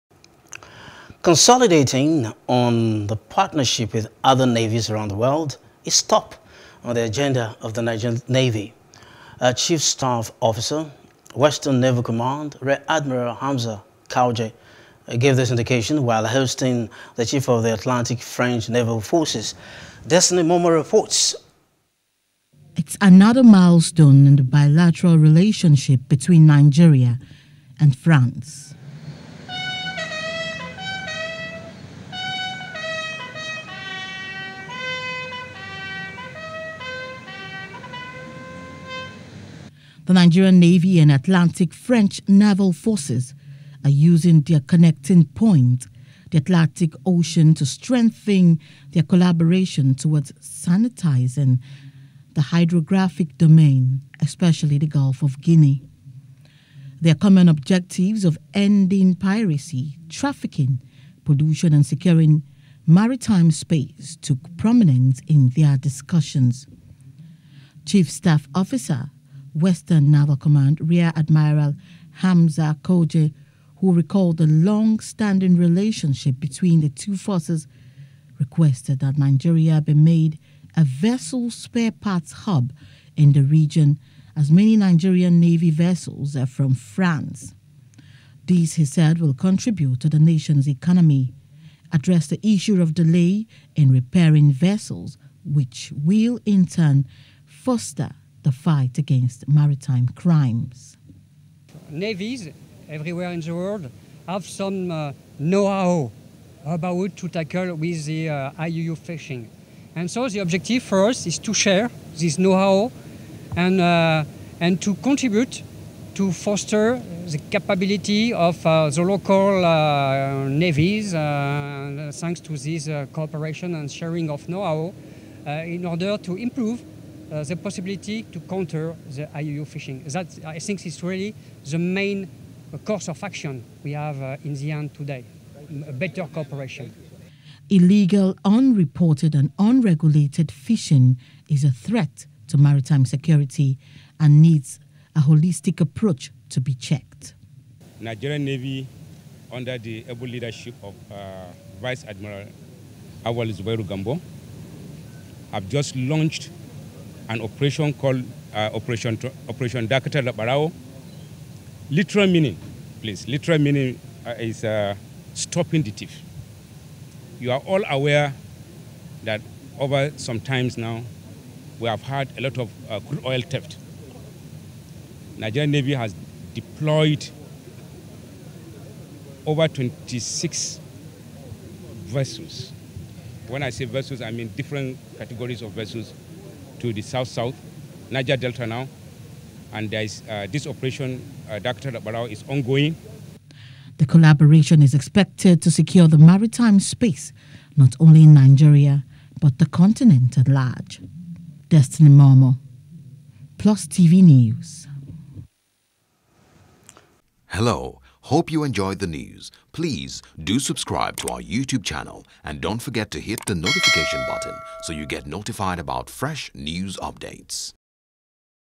Maritime: Nigerian Navy Partners France To Beef Up Waterways Security | NEWS
Consolidating on the partnership with other Navies around the world is top on the agenda of the Nigerian Navy.